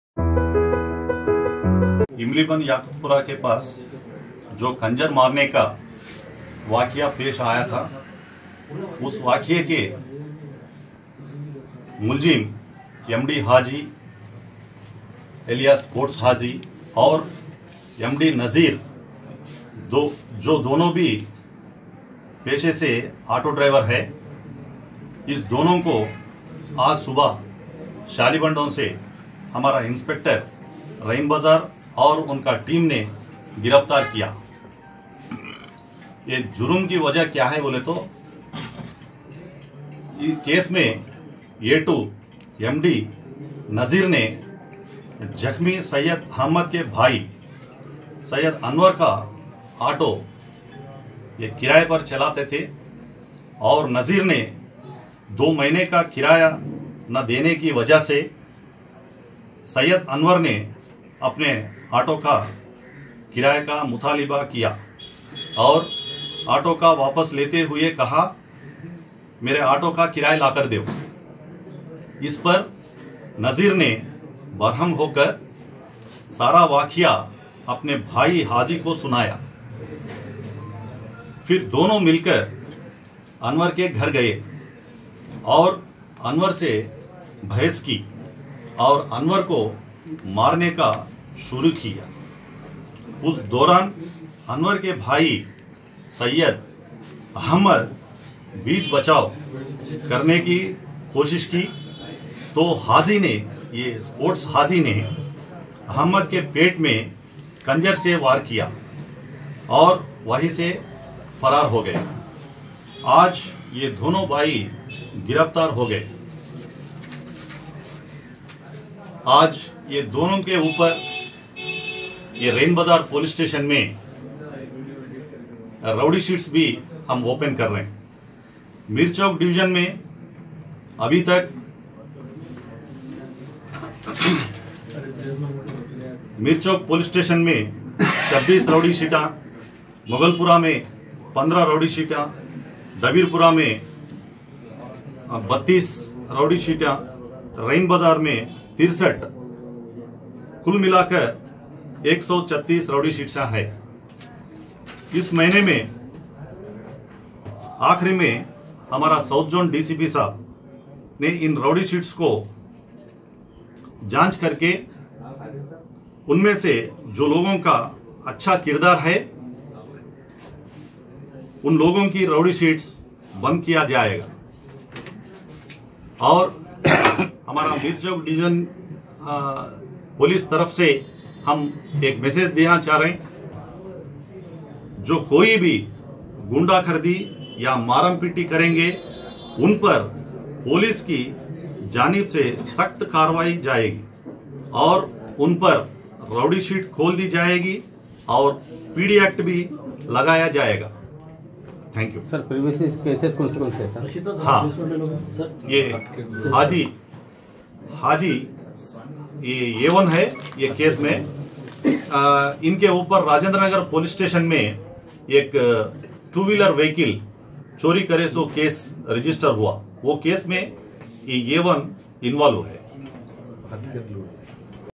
Attempt to murder case rain bazar accused will call rowdy sheet ..
News of crime rain bazar police today arrested 2 person who attacked a person 2 days before near ghouse cafe.imlibun.tiday acp produce before media criminals .inspector jaweed and team arrest report azmath Jaffery